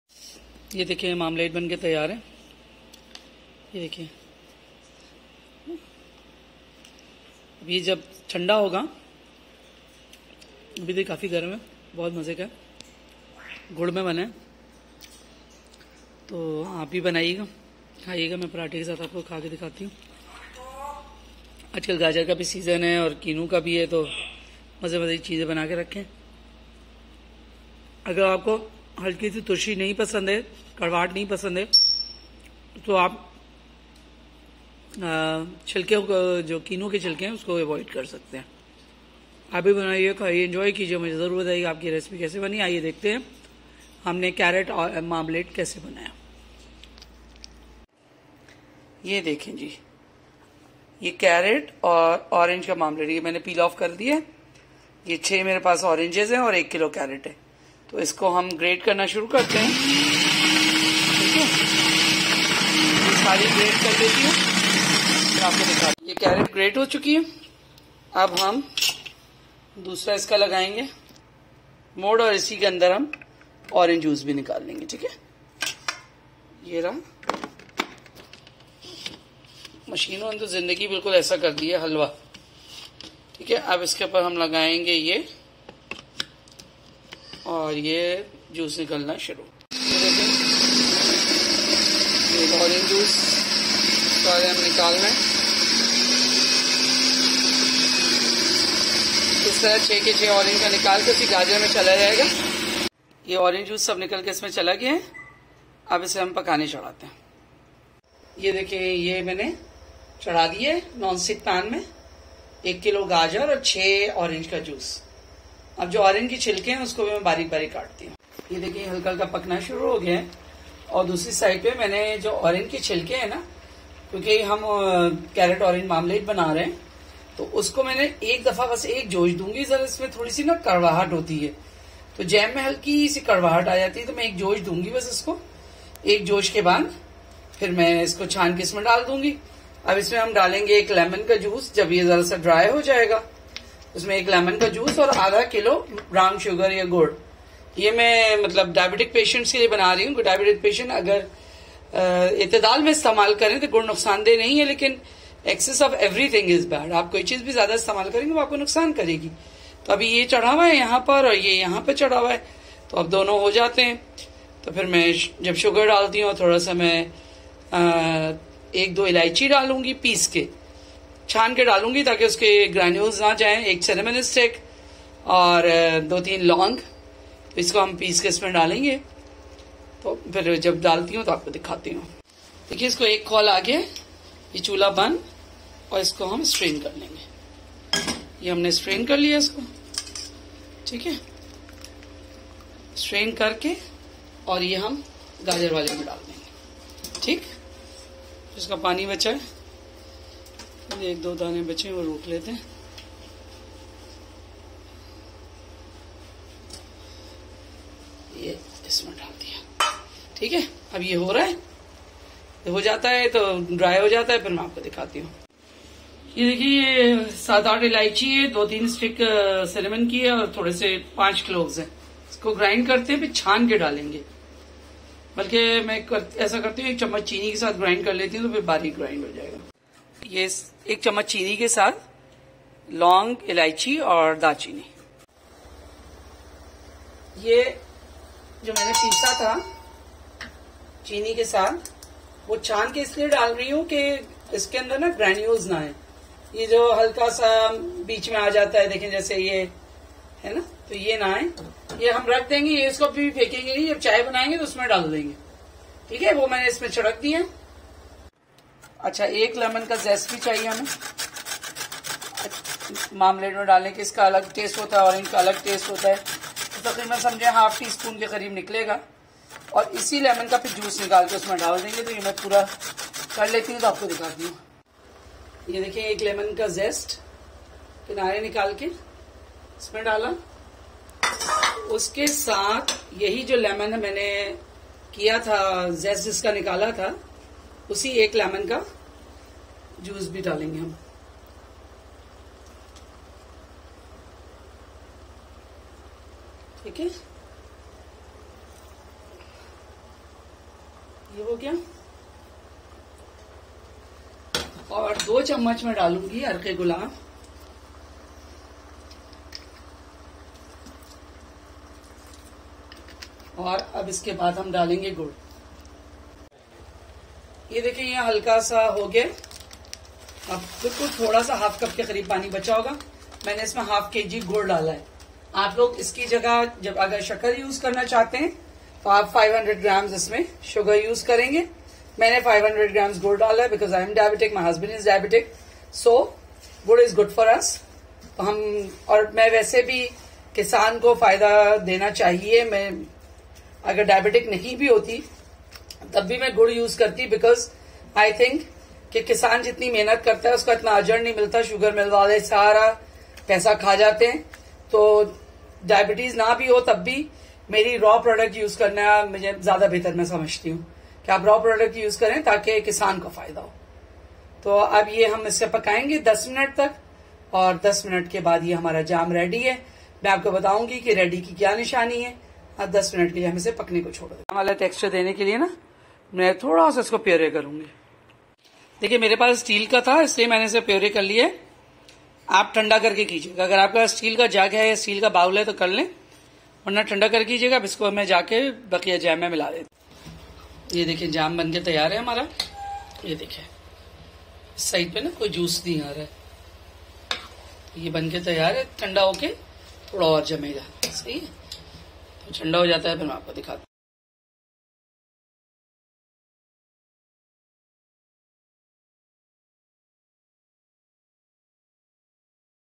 Carrot Orange marmalade
carrots 1 kg
orange 6
lemon 1 for zest and juice
jaggery or gurr 500 gms
small cardamom 7-8
cinnamon sticks 5
cloves 7
rose water 2 tbsp